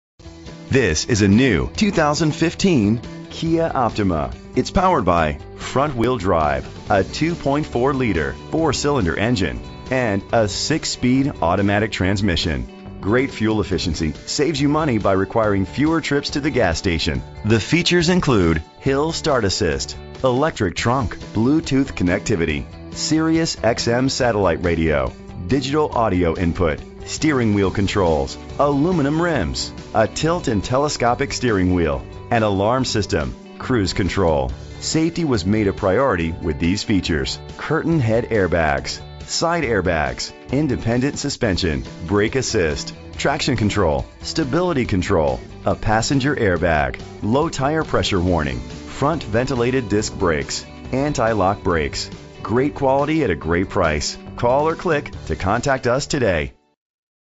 2015 Kia Optima - Willowbrook IL
Year: 2015
Make: Kia
Model: Optima
Trim: LX
Engine: 2.4 liter inline 4 cylinder 16 valve
Transmission: 6-Speed Automatic
Color: Ebony Black
Mileage: 5
Address:
7301 S. Kingery Hwy Rte 83